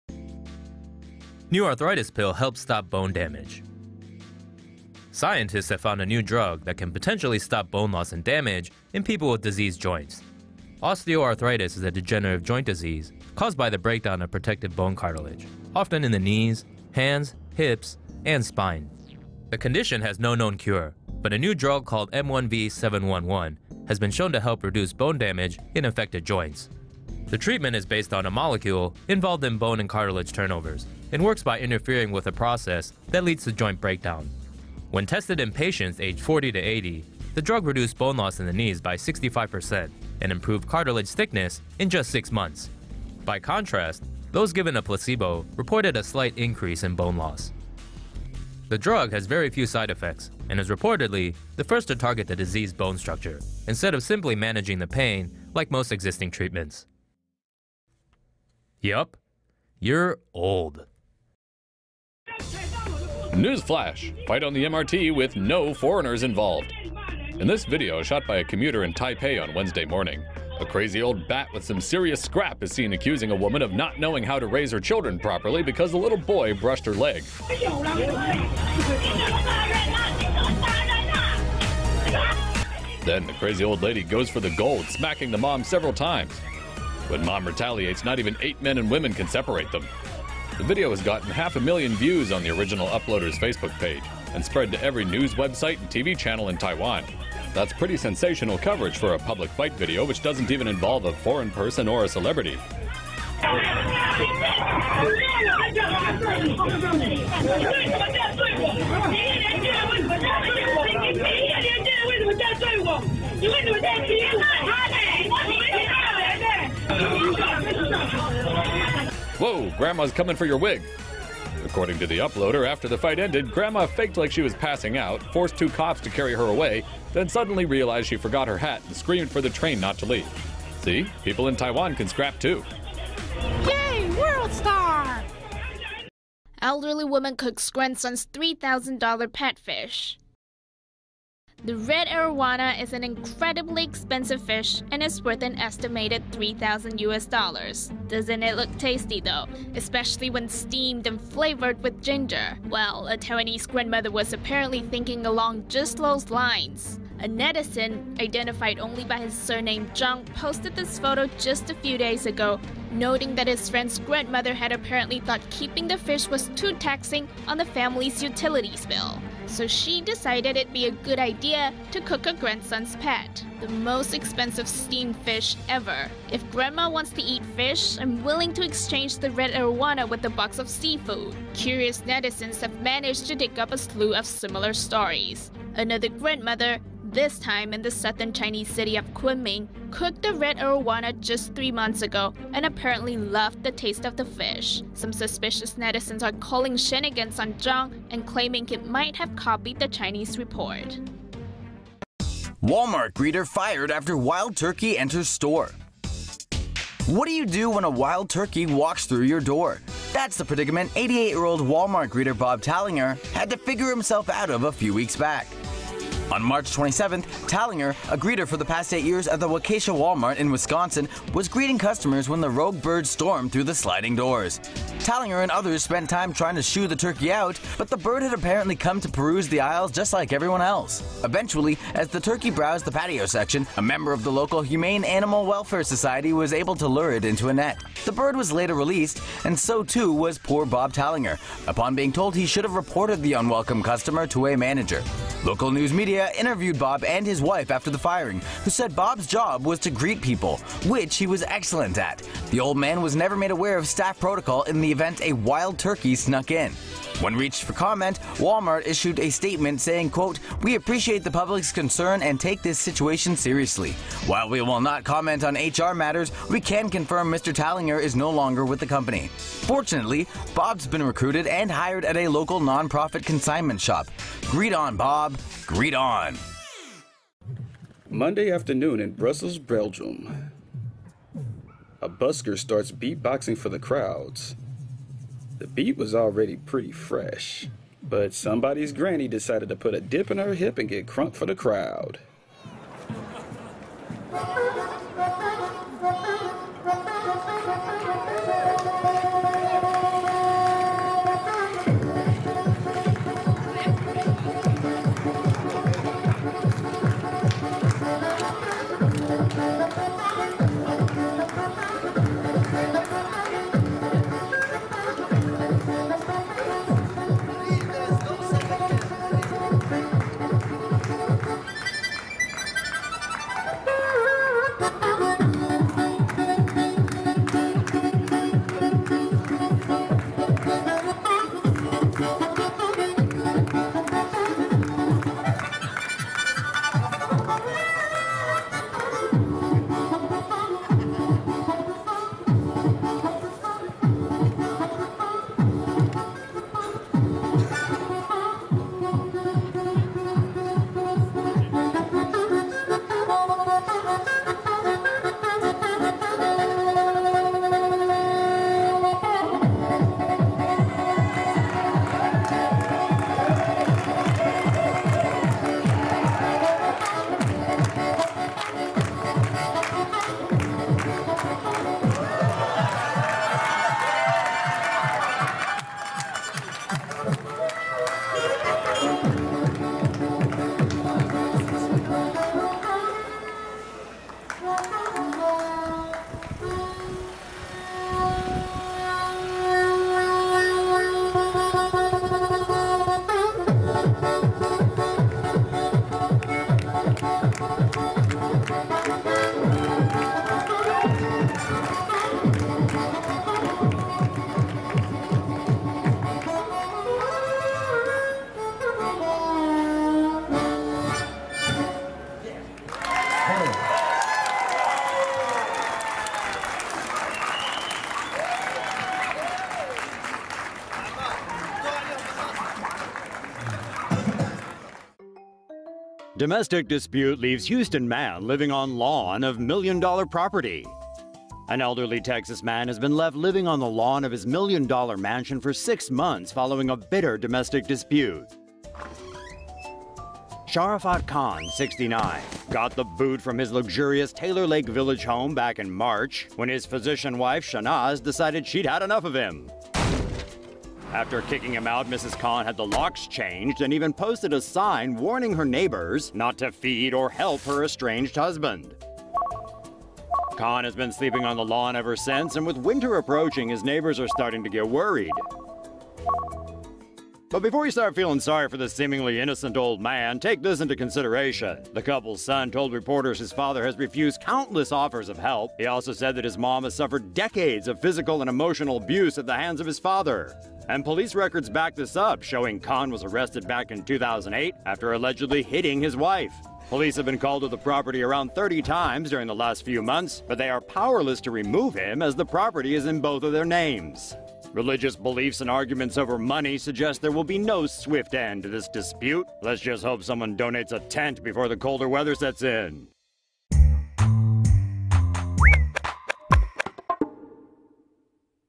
Arthritis treatment: New pill helps stop bone loss and damage caused by osteoarthritis - TomoNews
LEEDS, UNITED KINGDOM — Scientists have found a new drug that can potentially stop bone loss and damage in people with diseased joints.

The Daily Express reports that osteoarthritis is a degenerative joint disease caused by the breakdown of protective bone cartilage, often in the knees, hands, hips, and spine.

The condition has no known cure, but a new drug called M1V-711 has been shown to help reduce bone damage in affected joints.

The treatment is based on a molecule involved in bone and cartilage turnovers, and works by interfering with the process that leads to joint breakdown.

When tested in patients aged 40 to 80, the drug reduced bone loss in the knees by 65% and improved cartilage thickness in just six months. By contrast, those given a placebo reported a slight increase in bone loss.

According to lead investigator Philip Conaghan of the University of Leeds, the drug has very few side effects, and is reportedly the first to target the diseased bone structure, instead of simply managing pain like most existing treatments.